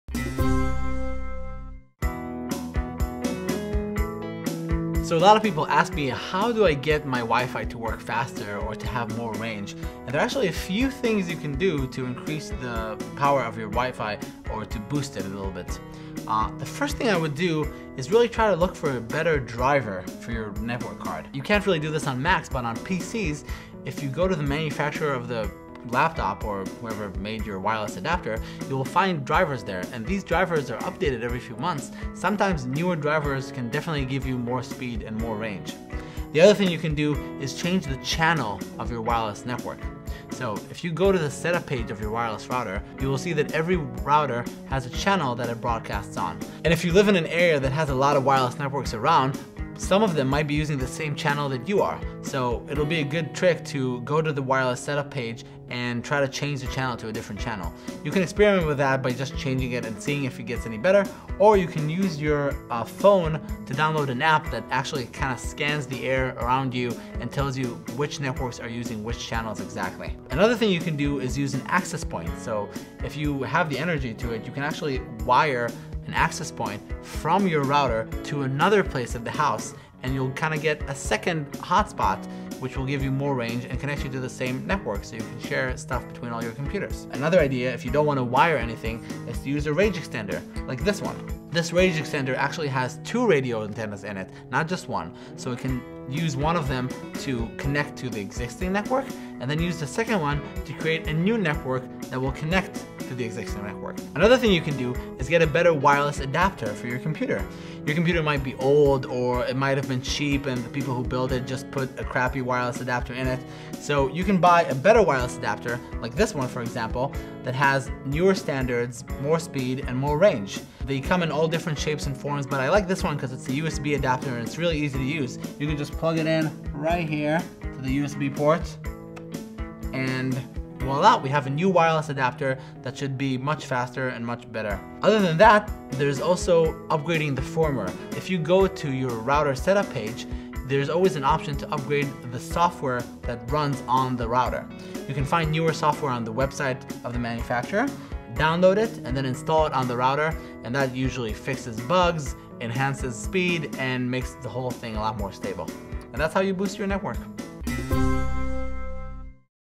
How to Boost Wi-Fi | Internet Setup
So a lot of people ask me, "How do I get my Wi-Fi to work faster or to have more range?" And there are actually a few things you can do to increase the powere of your Wi-Fi or to boost it a little bit. The first thing I would do is really try to look for a better driver for your network card. You can't really do this on Macs, but on PCs if you go to the manufacturer of the laptop or whoever made your wireless adapter, you will find drivers there. And these drivers are updated every few months. Sometimes newer drivers can definitely give you more speed and more range.

The other thing you can do is change the  channel of your wireless network. So if you go to the setup page of your wireless router, you will see that every router has a channel that it broadcasts on. And if you live in an area that has a lot of wireless networks around, some of them might be using the same channel that you are. So it would be a good trick to go to the wireless setup page and try to change the channel to a different channel. You can experiment with that by just changing it and seeing if it gets any better, or you can use your phone to download an app that actually kind of scans the air around you and tells you which networks are using which channels exactly.

Another thing you can do is use an access point. So if you have the energy to do it, you can actually wire an access point from your router to another place at the house, and you'll kind of get a second hotspot which will give you more range and connects you to the same network so you can share stuff between all your computers. Another idea, if you don't want to wire anything is to use range extender like this one. This range extender actually has two radio antennas in it, not just one, so it can use one of them to connect to the existing network, and then use the second one to create a new network that will connect to the existing network.

Another thing you can do is get a better wireless adapter for your computer. Your computer might be old, or it might have been cheap, and they people who built it just put a crappy wireless adapter in it. So you can buy a better wireless adapter, like this one for example, that has newer standards, more speed, and more range. They come in all different shapes and forms, but I like this one because it's a USB adapter, and it's really easy to use. You can just plug it in right here to the USB port, and voila, we have a new wireless adapter that should be much faster and much better.

Other than that, there's also upgrading the firmware. If you go to your router setup page, there's always an option to upgrade the software that runs on the router. You can find newer software on the website of the manufacturer, download it, and then install it on the router, and that usually fixes bugs, enhances speed, and makes the whole thing a lot more stable. And that's how you boost your network.